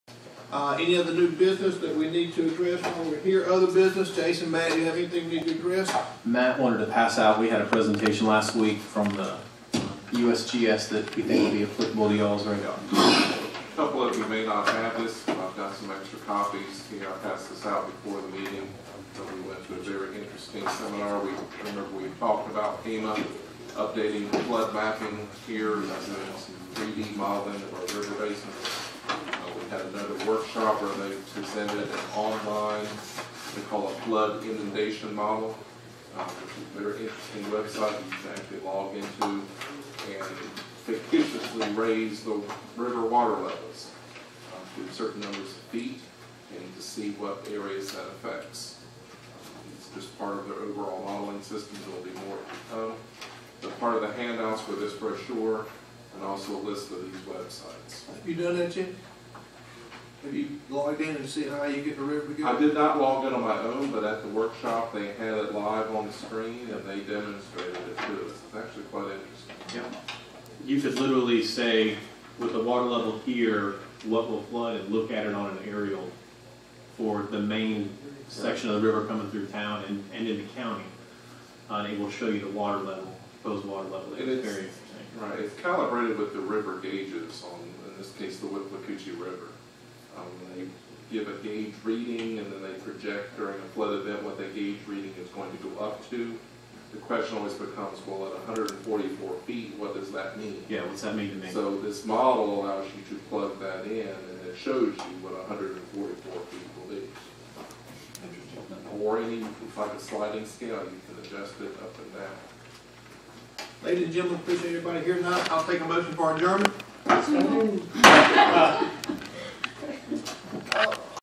6. Other Business - Flood Mapping @ GLPC - 2018-05-29
This was actually the most interesting bit of the meeting as staff talked about the new flood mapping tools that are available.